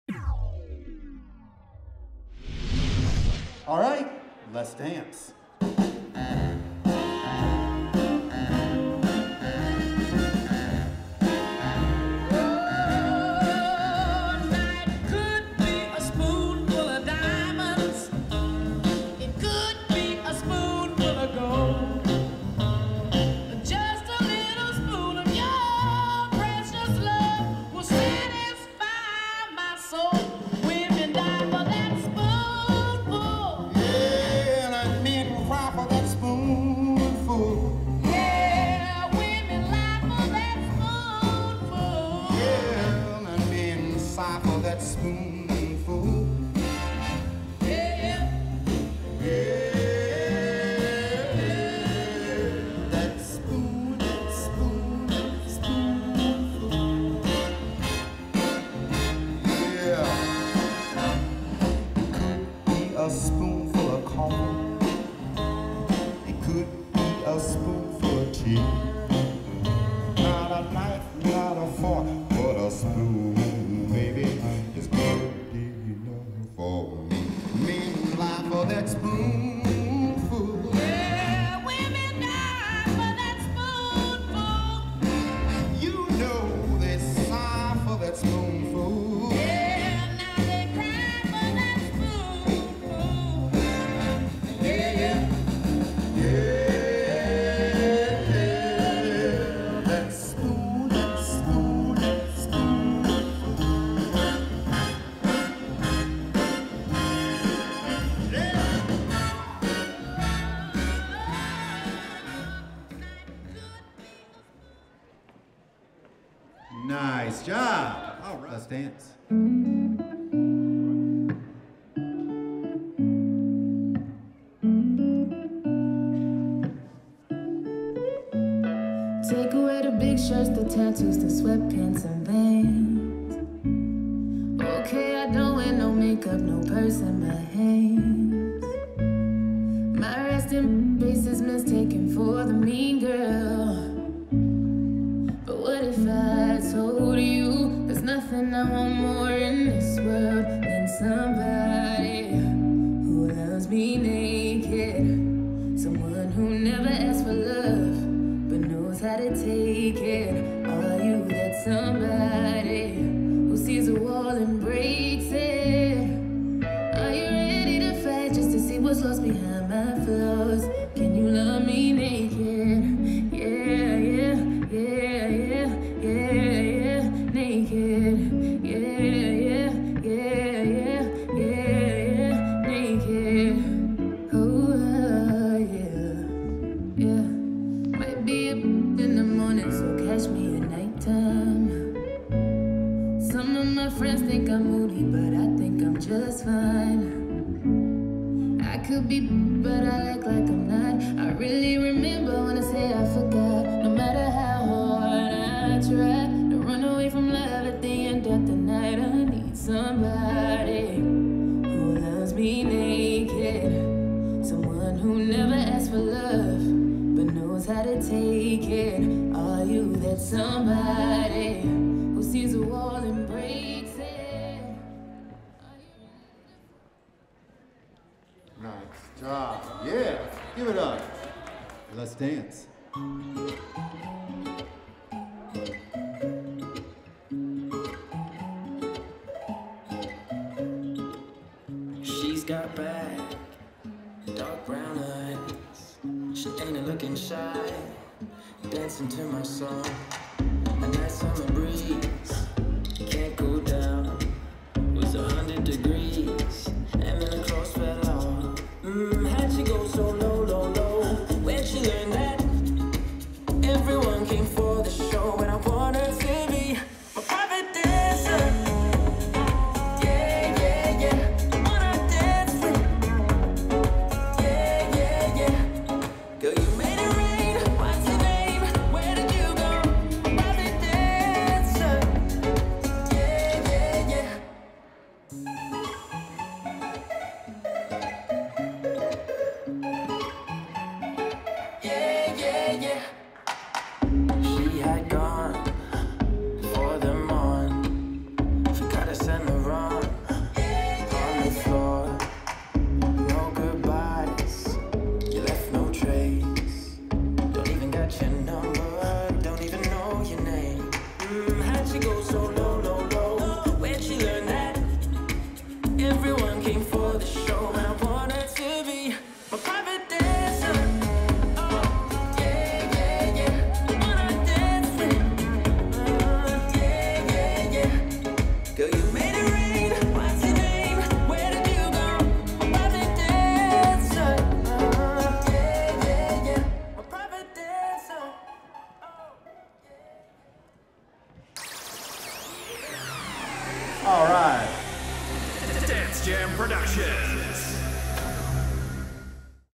MADjam 2022 Intermediate Strictly Swing
Recorded at the 2022 Mid Atlantic Dance Jam. MADjam is the largest West Coast Swing party in the world. It features contests for amateurs and professionals, dozens of workshops and amazing social dancing that brings together 100s of dancers from around the world on the first weekend in March. It is hosted in the Washington DC Area by Dave Moldover's Dance Jam Productions.

Video recording and editing by Moments of Dance LLC